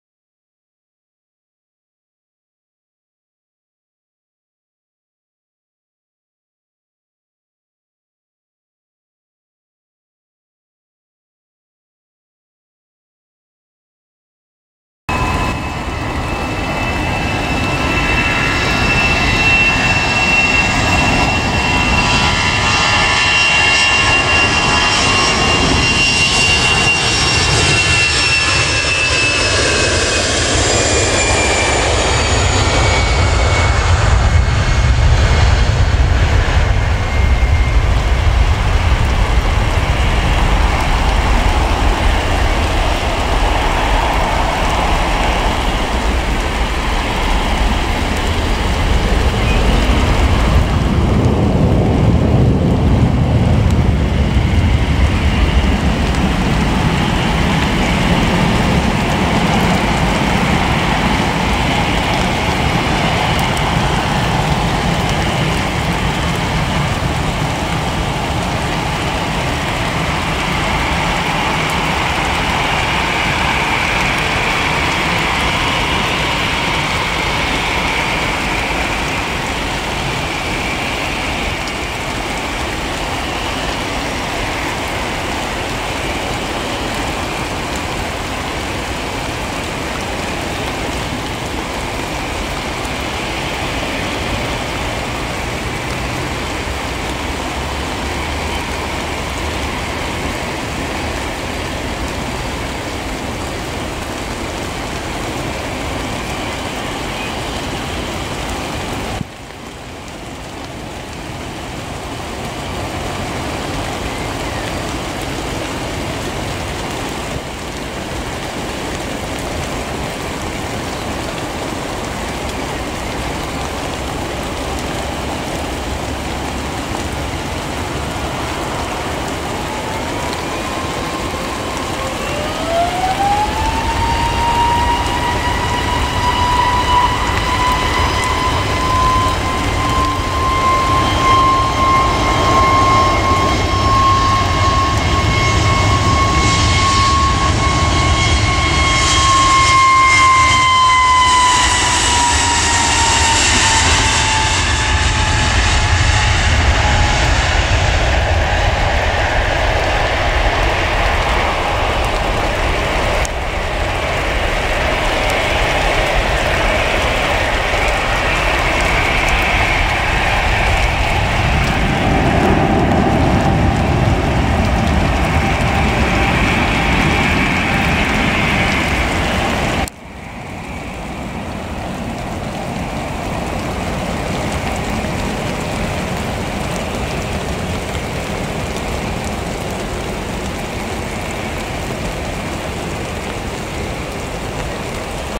Jet2 G-SUNB Airbus A321Neo wet takeoff from Manchester Airport
A quick video, one of Jet2 latest aircraft, there new Airbus A321Neo Registered as G-SUNB wearing Jet2Holidays livery taking off from 23R in the rain from Manchester Airport.